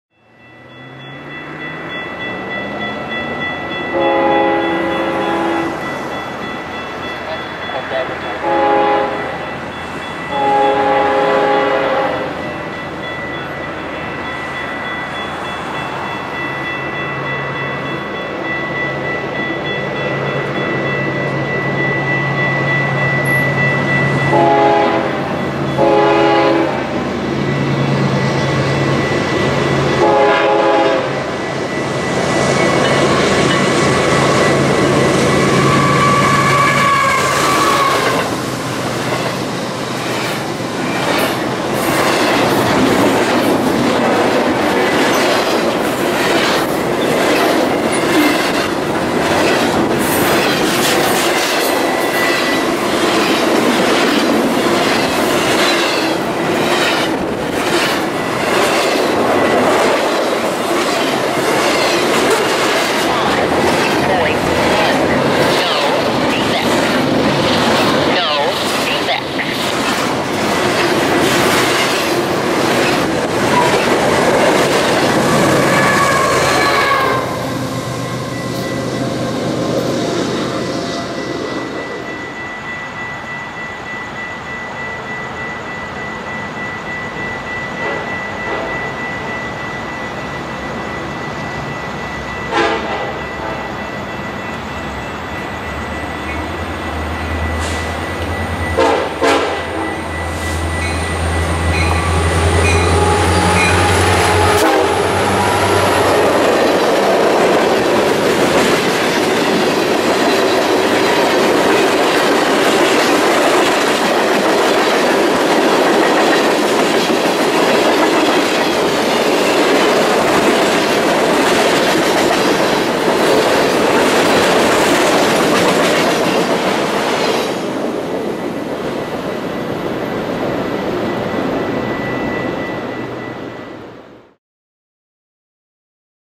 HD- 2 Freights In The Valley: 9/23/11
Here's a Doublestack in Sun Valley, and a Manifest Local in Downtown Burbank. Enjoy!